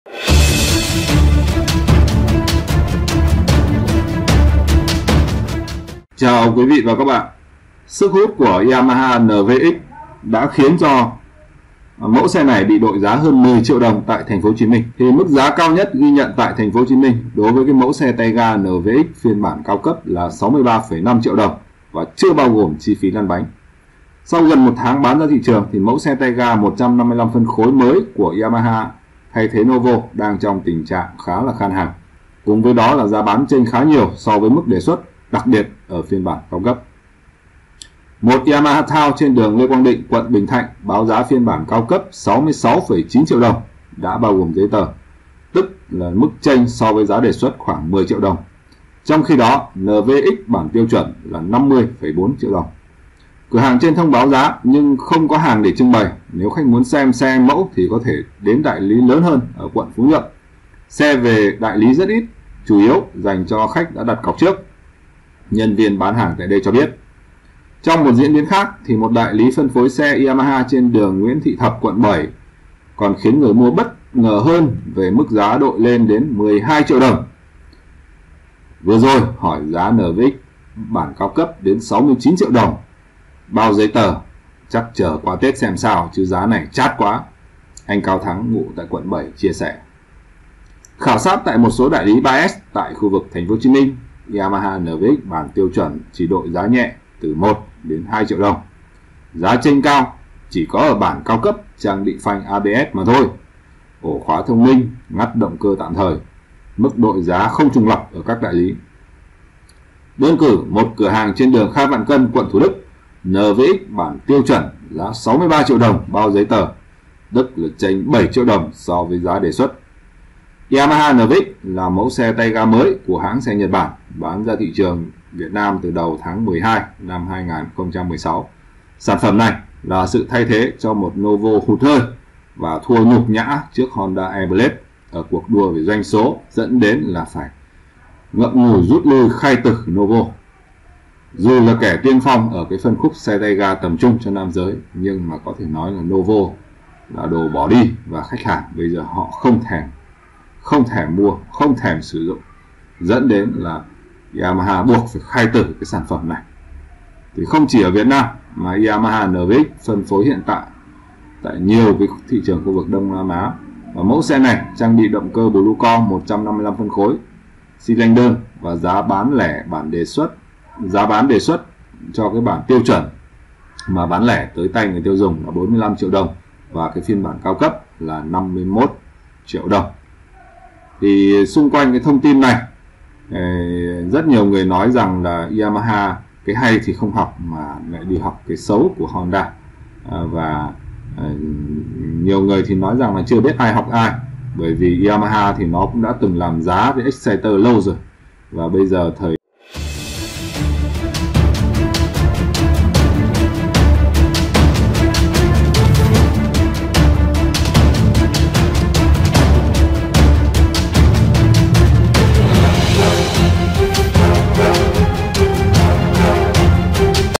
Yamaha NVX 155 có giá bán ảo tung chảo tại TpHCM
Yamaha NVX 155 có giá bán ảo tung chảo tại TpHCM, theo đó giá xe NVX 155 bản đặc biệt có giá 63,5 triệu đồng chưa bao gồm chi phí đăng ký ra bảng số. Như vậy tại thị trường Tp HCM xe tay ga mới NVX 155cc bị đội giá cao hơn thị trường Hà Nội.